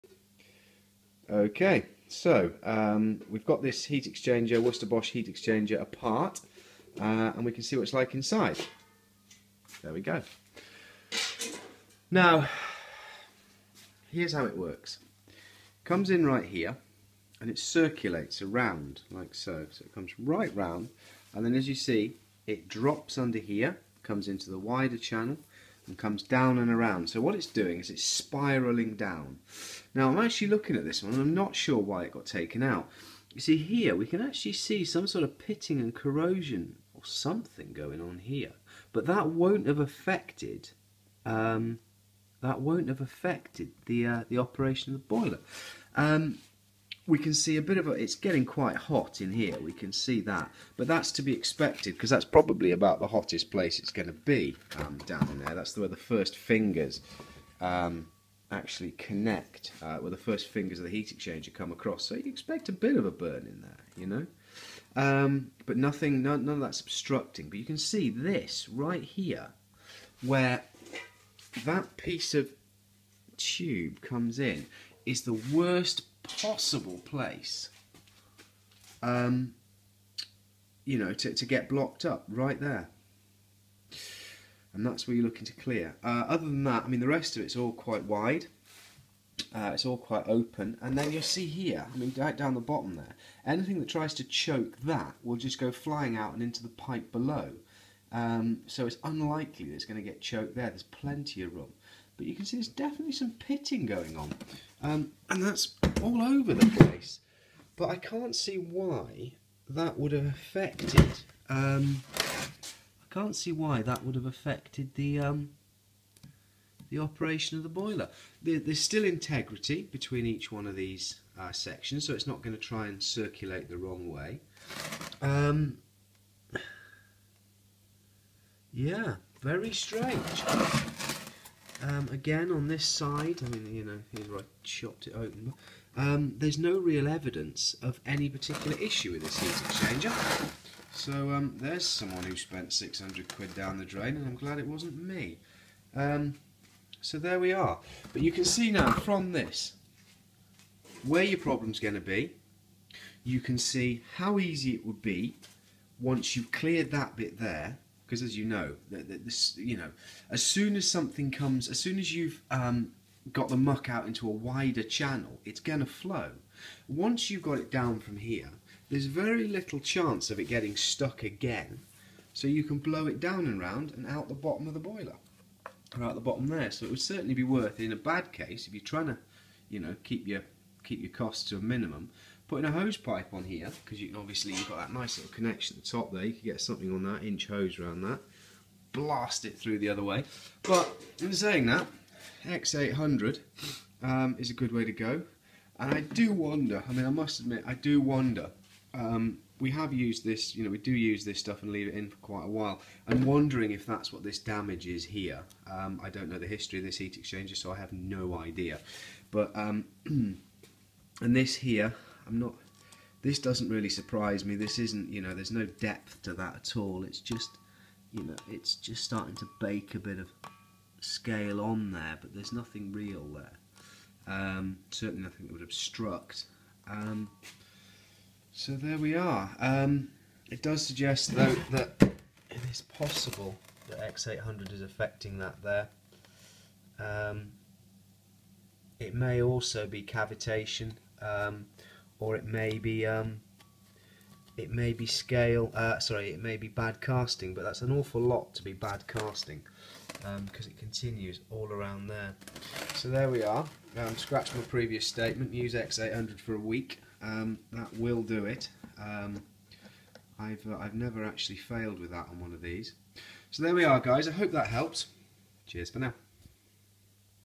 Worcester heat cell internal view - problem solving
Look inside the Worcester bosch round heat cell